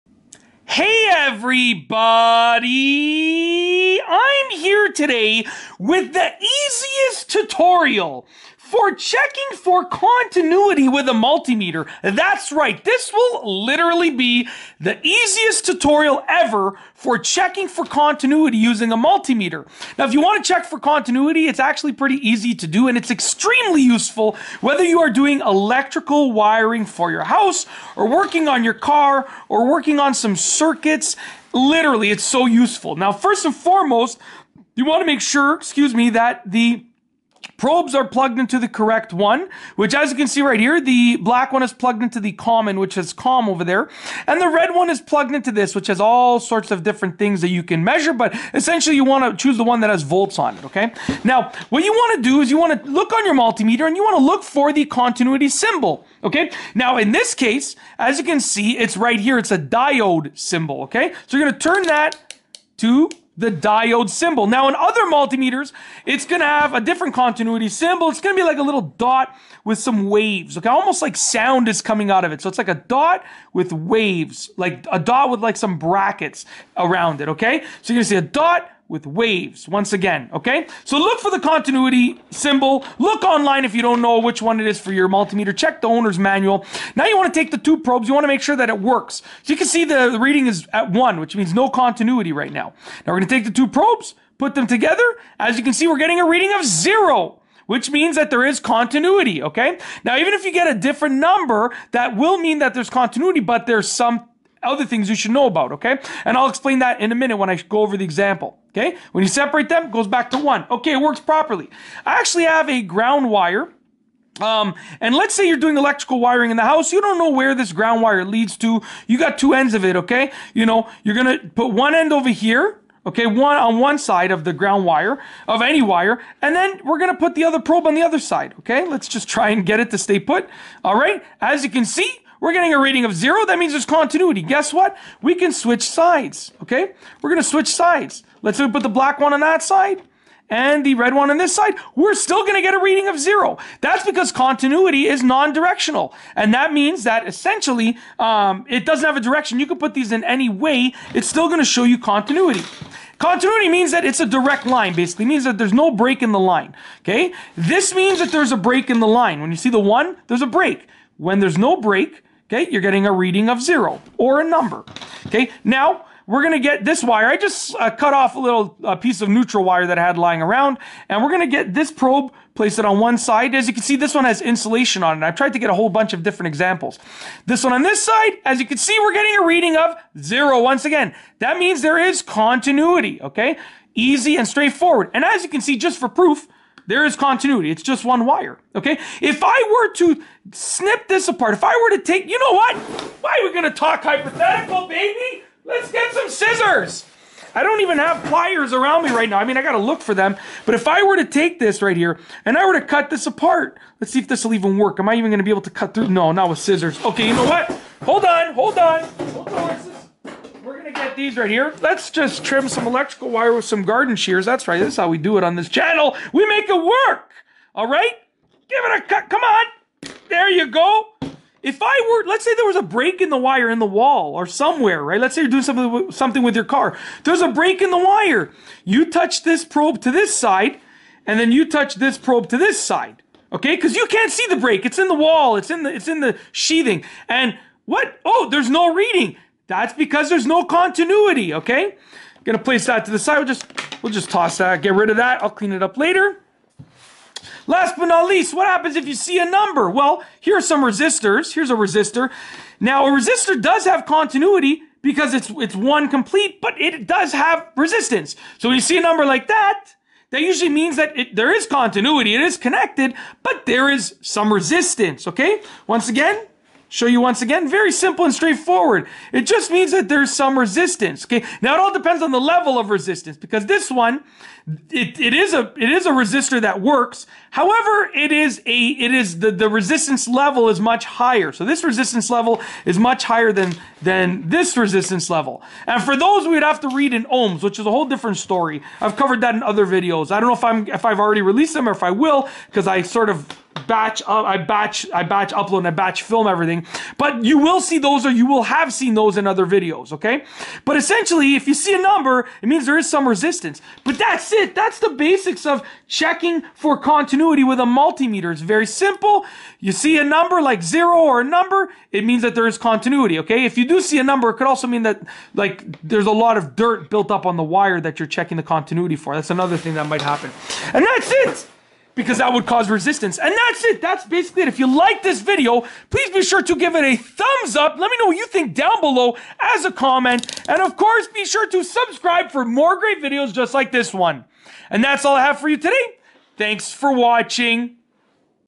Checking For Continuity With A Multimeter-Easiest Tutorial
In this video, I go over the basic steps for checking for continuity with a multimeter. if you have a multimeter and you want to see if a wire is connected, and you have access to both of the sides, you can check for continuity. This is useful in many different situations, but primarily used in automotive wiring, household wiring, and consumer electronic wiring. It is a common method that is used to solve problems and get to the bottom of what is causing something to occur within a given set of wiring!